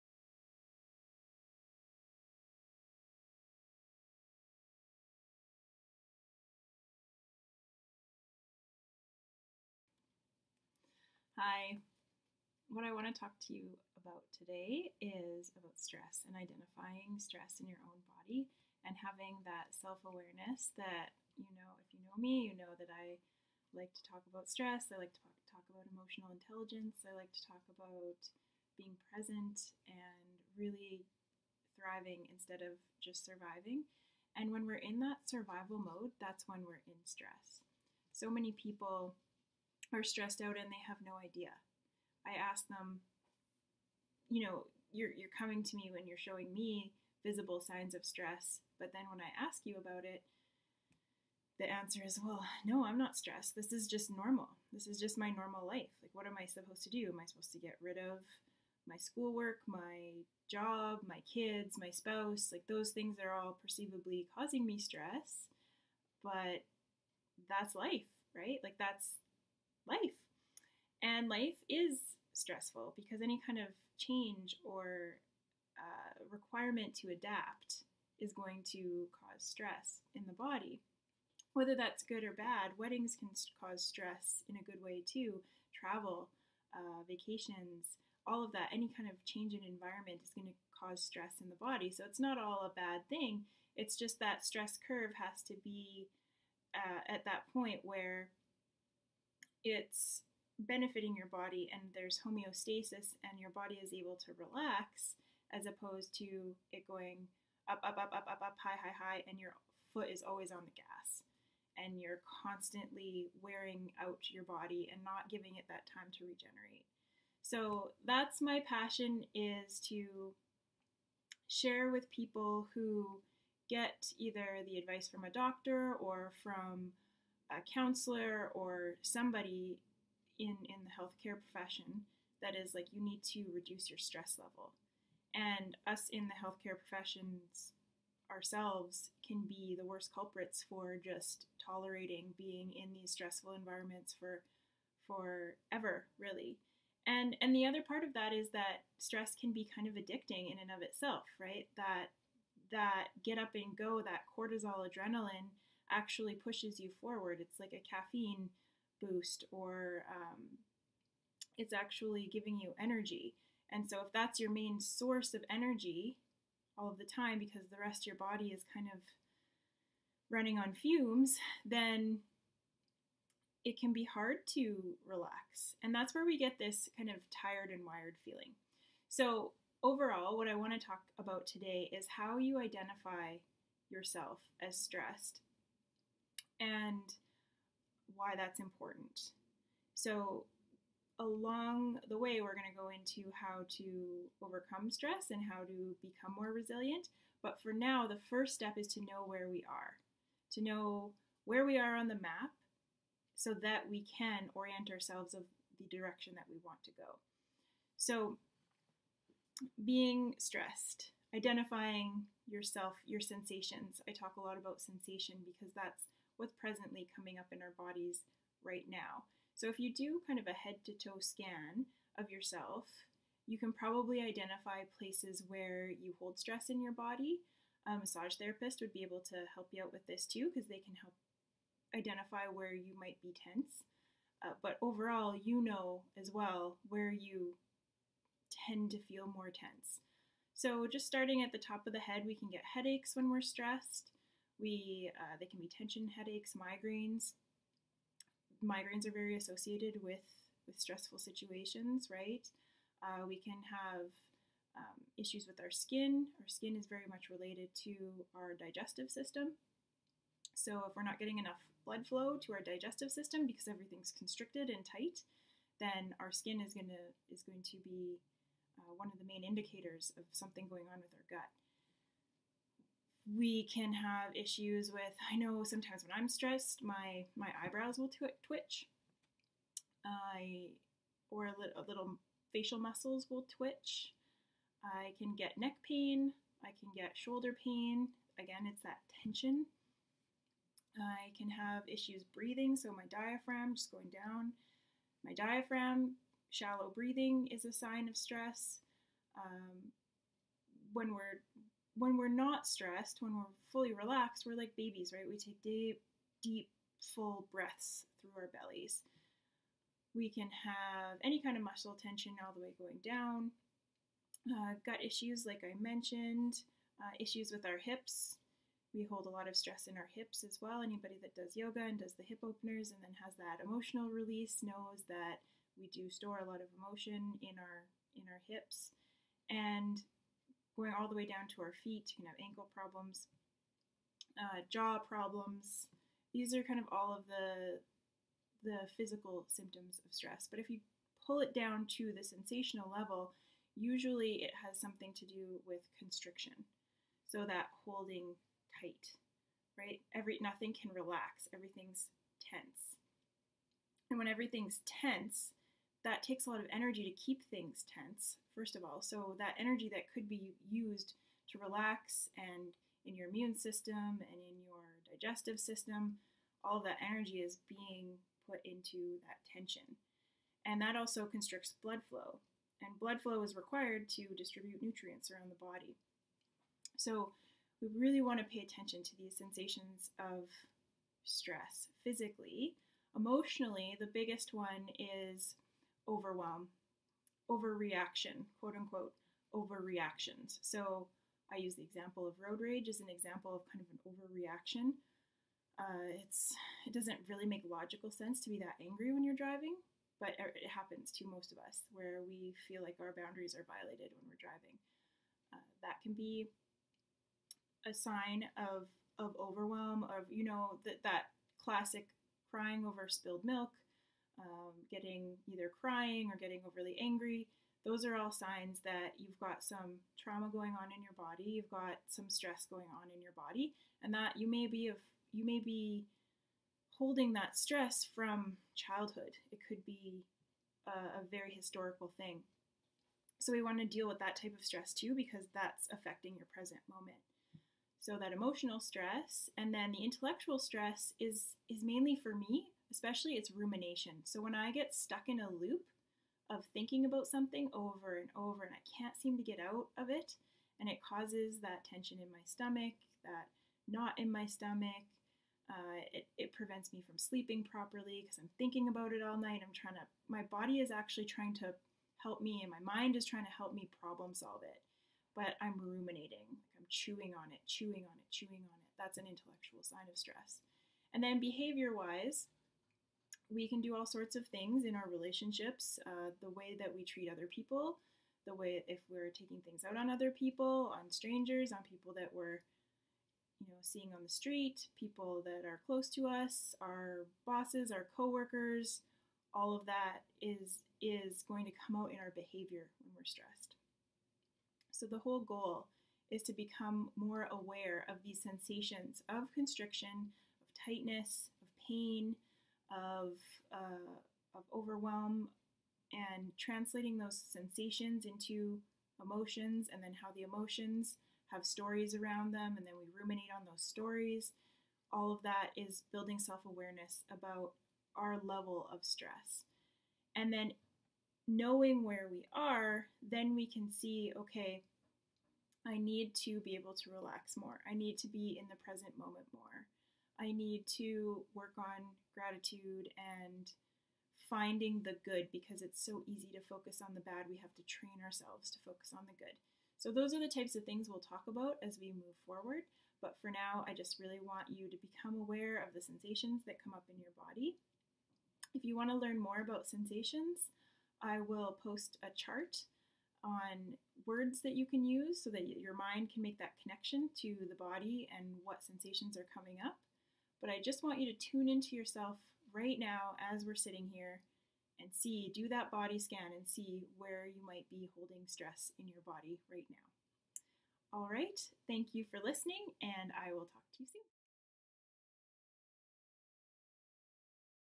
Signs of Stress.
Signs of Stress. Healing Workplace Stress. Identifying sensations of stress. Self-awareness.
Thriving vs surviving.
Knowing where we are so that we can head in the direction we want to go!

Becky Palmer
Brilliance Health Coaching